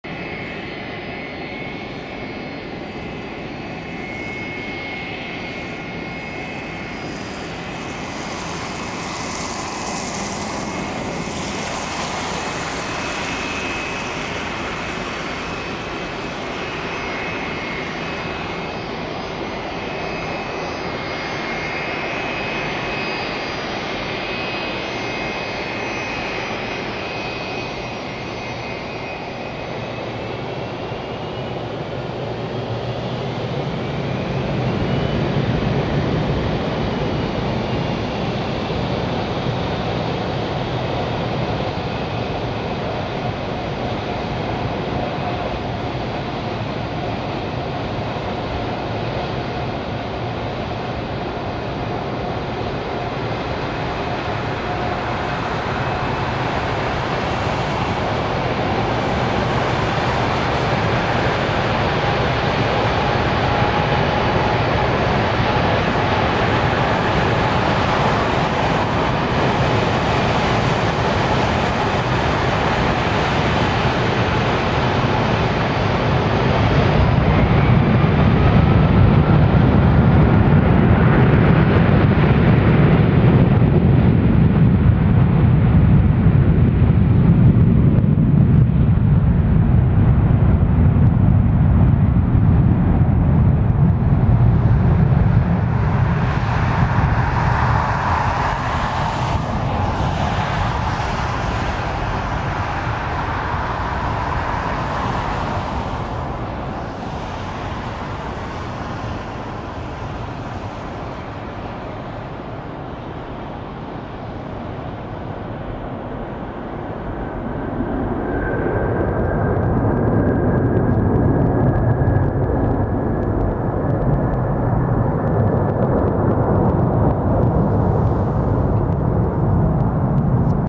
Allegiant Air 757-200 Departure from Spokane, WA (KGEG)
AAY1065
KGEG-PHNL
Boeing 757-204